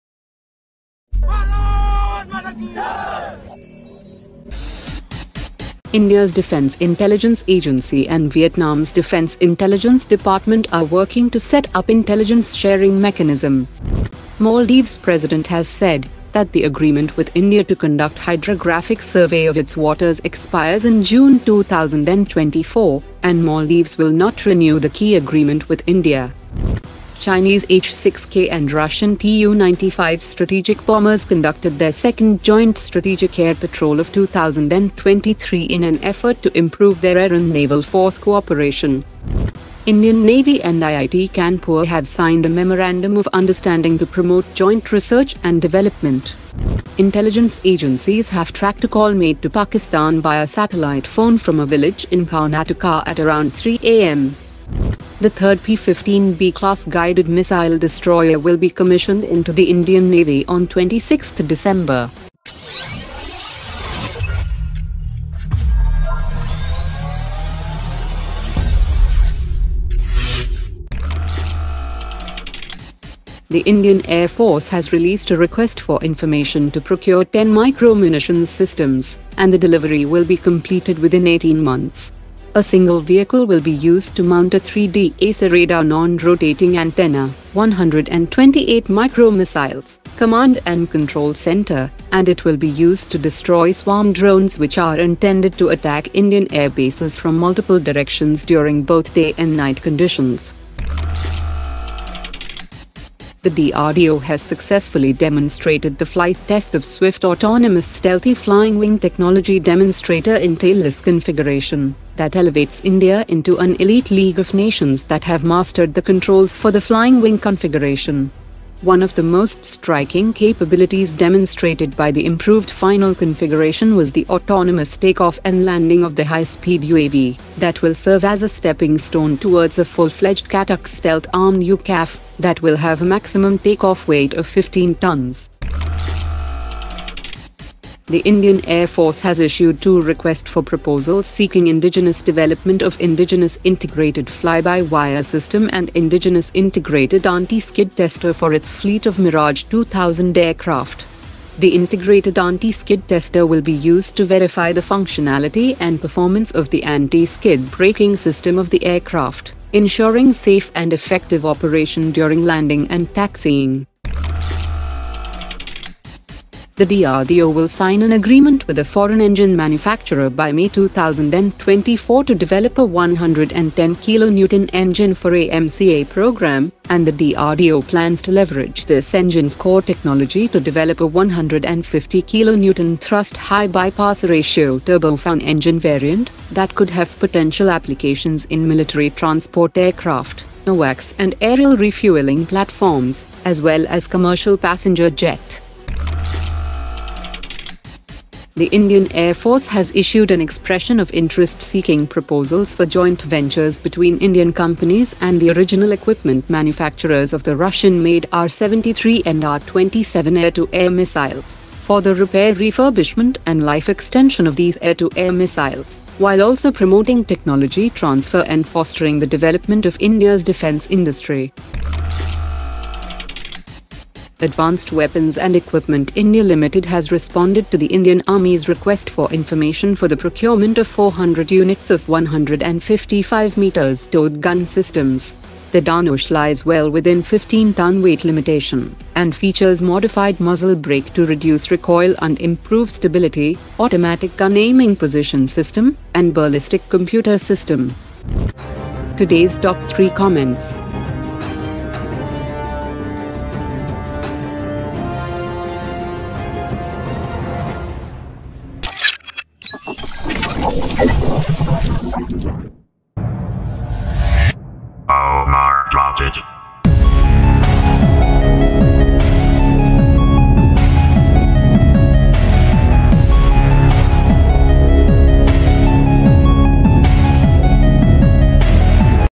Indian Defence Updates : New 150 Kn Engine,UCAV Flight Test,10 Anti Swarm Order,Mirage-2000 Upgrade
🔴 Fast Headlines :
1. India & Vietnam to set up Intelligence sharing mechanism.
2. Maldives Declines Renewal of Hydrographic Survey Agreement with India.
3. China Russia joint air patrol improves air-sea co-ordination.
4. Indian navy & IIT Kanpur join forces for joint research partnership.
5. Authorities track satellite phone call to Pakistan from Karnataka district
6. INS Imphal to be commissioned by 26th December

🔴 Detailed Updates :
7. IAF to procure 10 Mobile Munitions Based
8. DRDO conducts successful flight test of swiftucav
9. IAF seeks indigenous solutions for Mirage-2000 upgrades
10. High Thrust Engine for AMCA will have a spinoff 150 Kn engine to power transport aircraft
11. IAF seeks JV for Repair Refurbishment & Life Extention of Russian A2A Missiles
12. AWEIL responds to Army's RFI for 400 Towed Gun systems